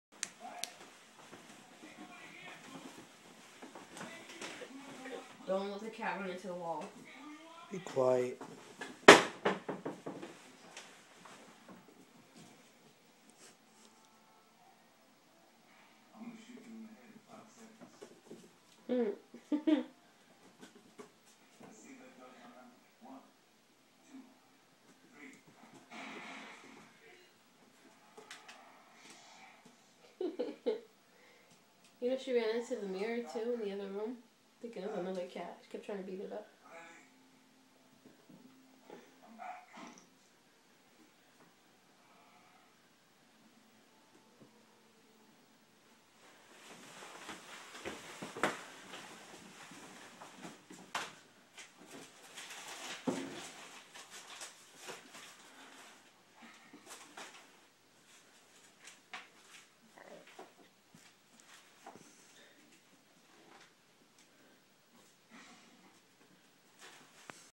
Fucking around with the cat
Typical retard cat runs after laser and thinks it could catch it.